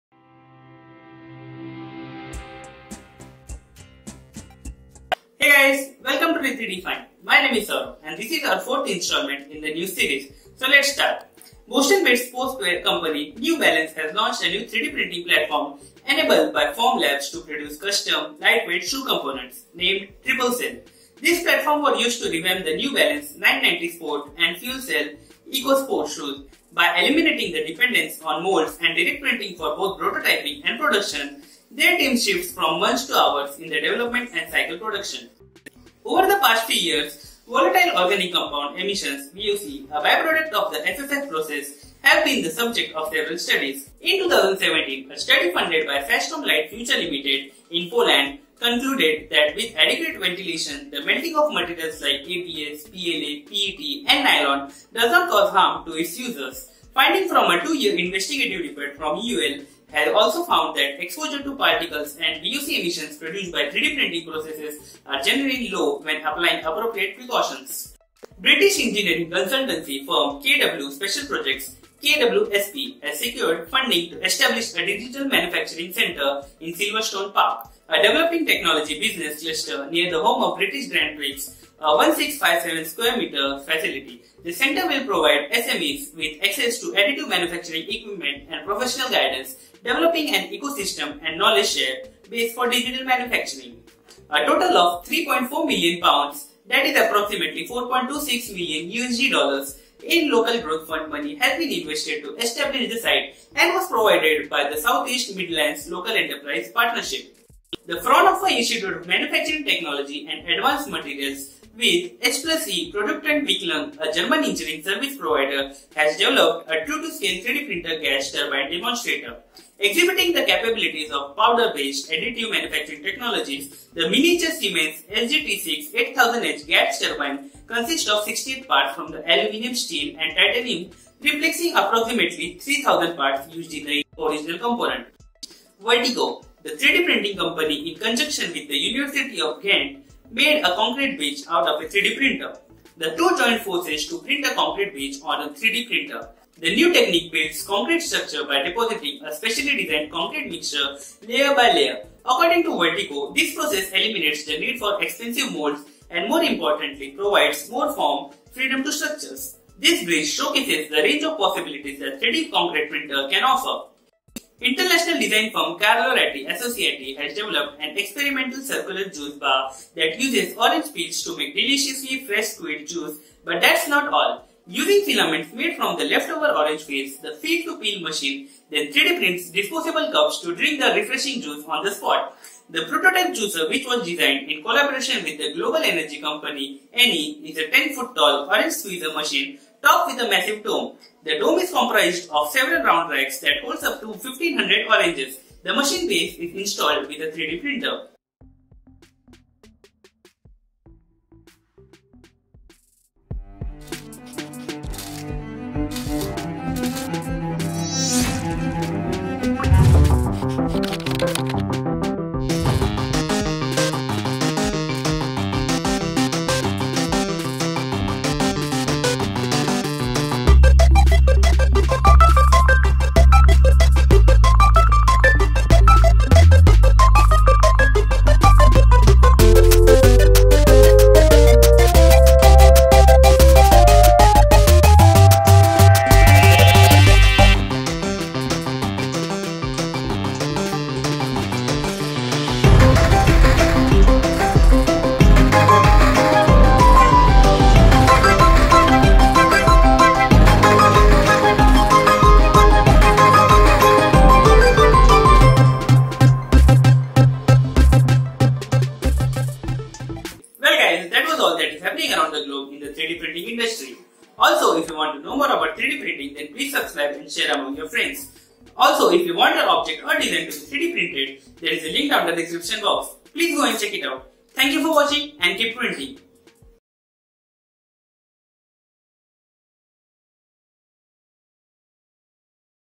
4News | Orange Plum Filament | 3D Printed Concrete Bridge | 3D Printed GAS TURBINE | 3D ForeFoot
At Re3Define - We Discuss and Create everything related to 3D printing

If you want your design to be 3d printed please fill up this google form

Or